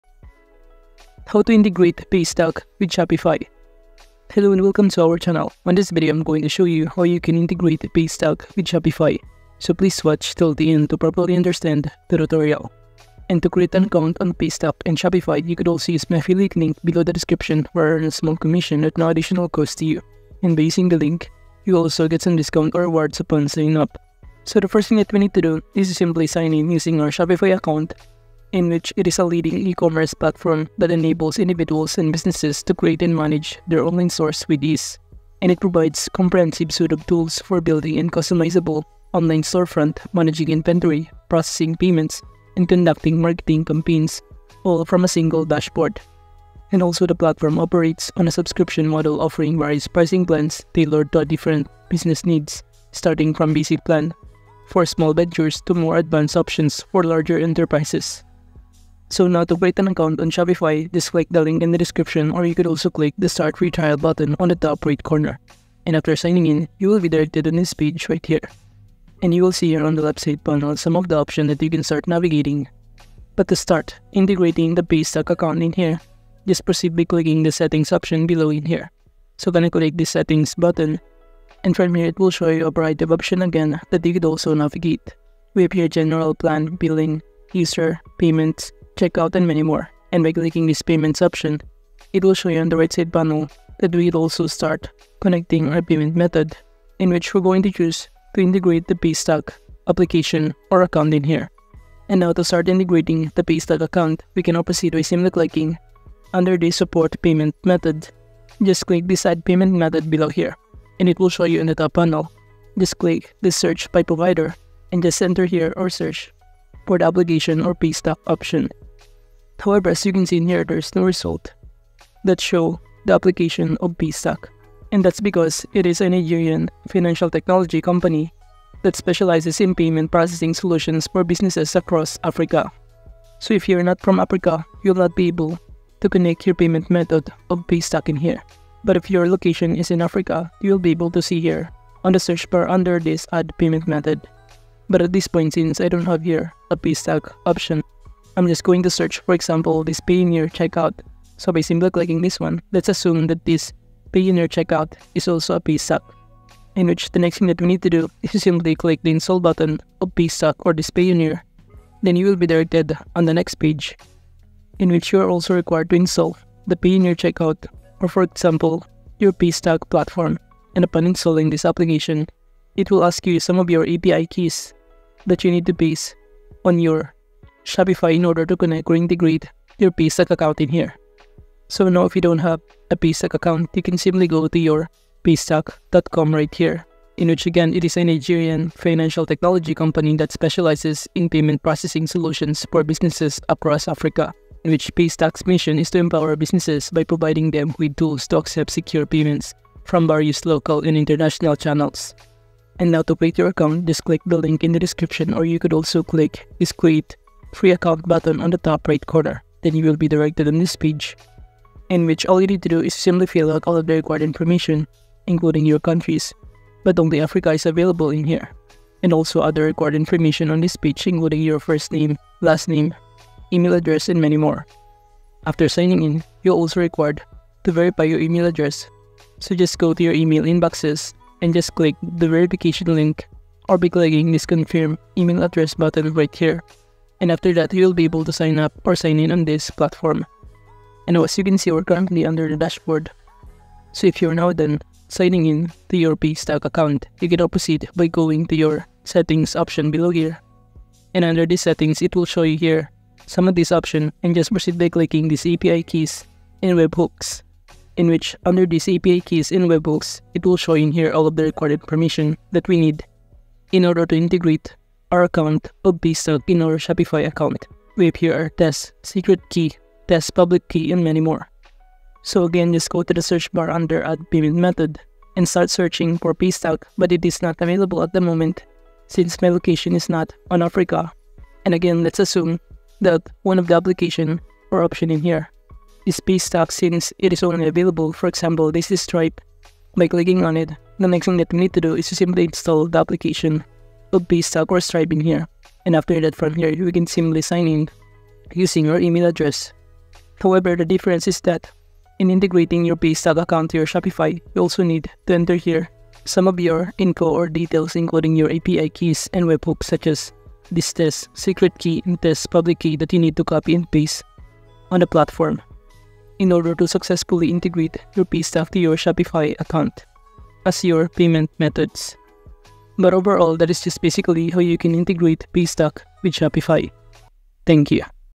How to Integrate Paystack With Shopify | Add Paystack to Shopify (2025)
In this tutorial we have shown how to add paystack to shopify / integrate paystack on your website